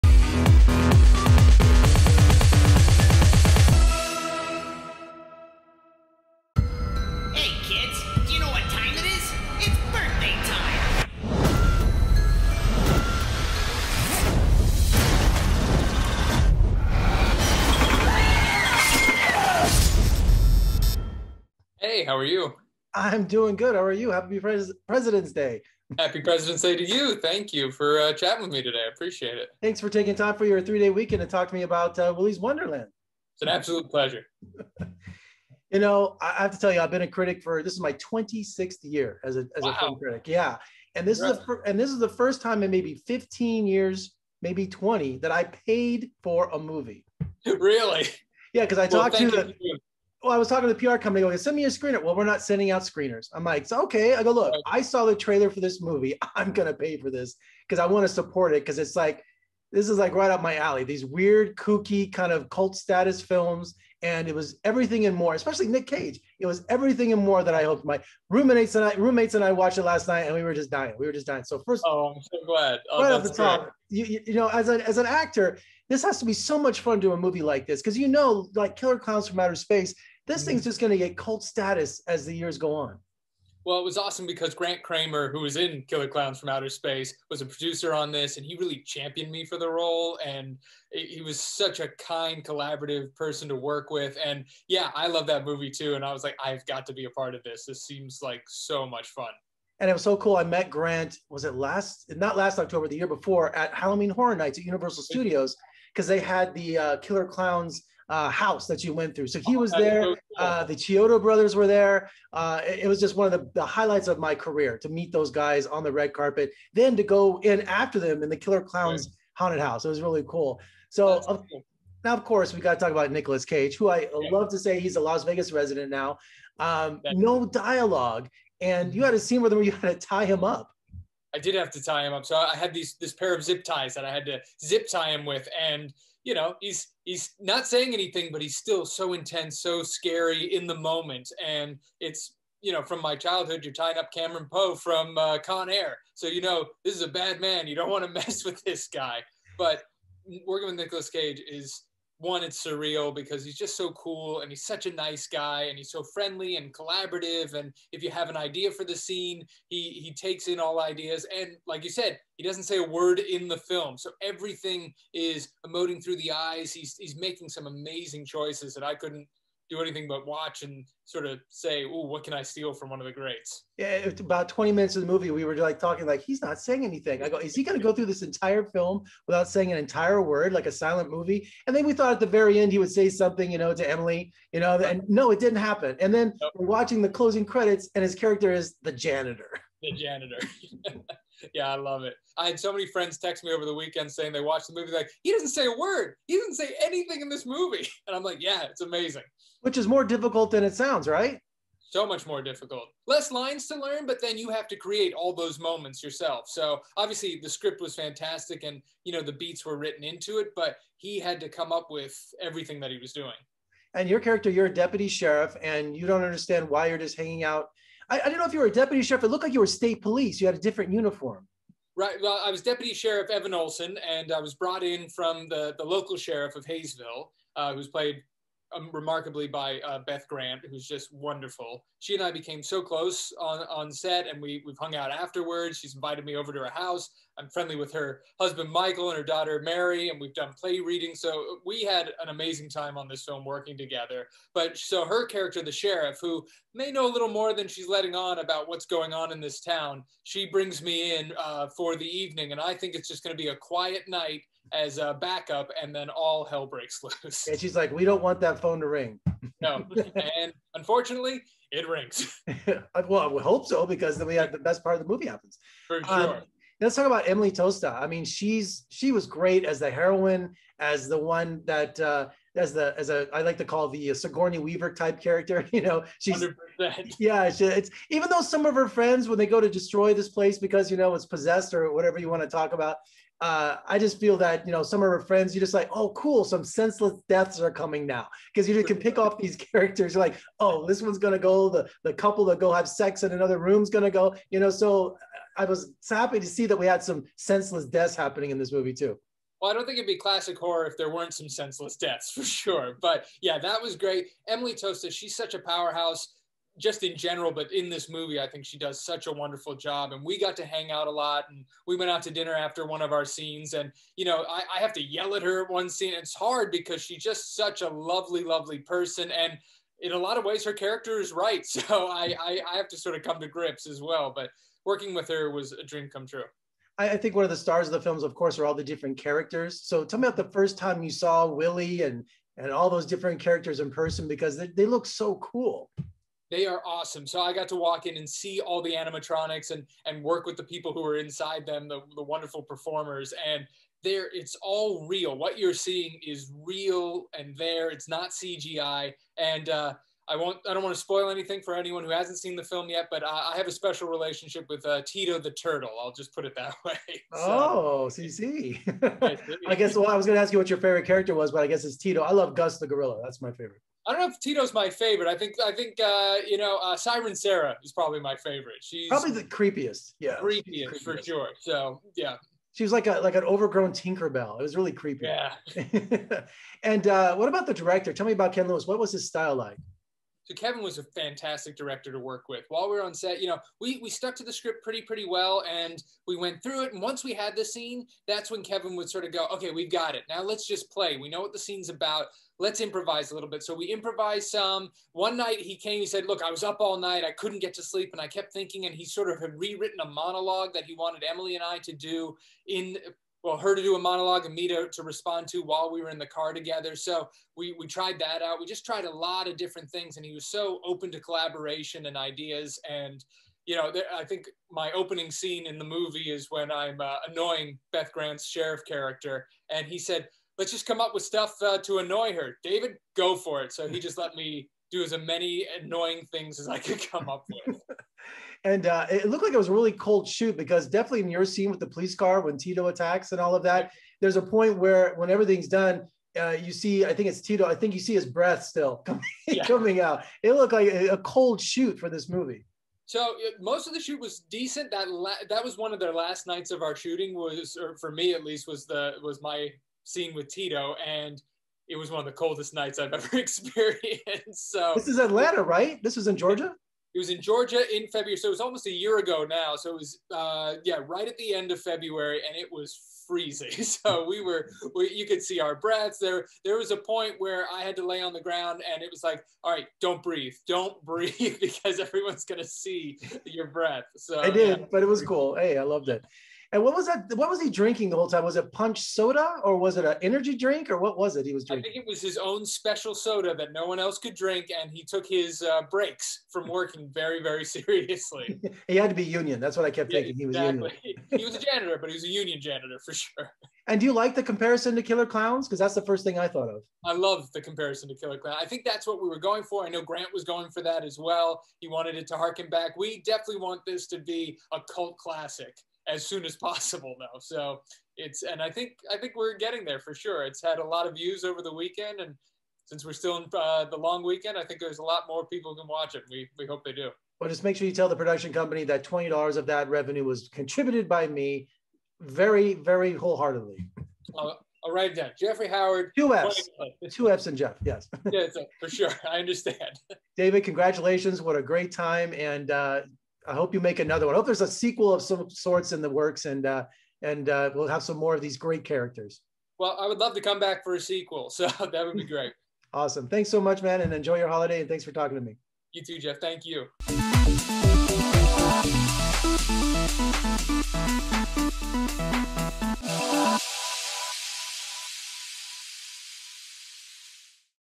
David Sheftell Interview - Willy's Wonderland
Vegas Film Critic (Jeffrey K. Howard) speaks with Actor David Sheftell for his new horror film, Willy's Wonderland - now available on VOD!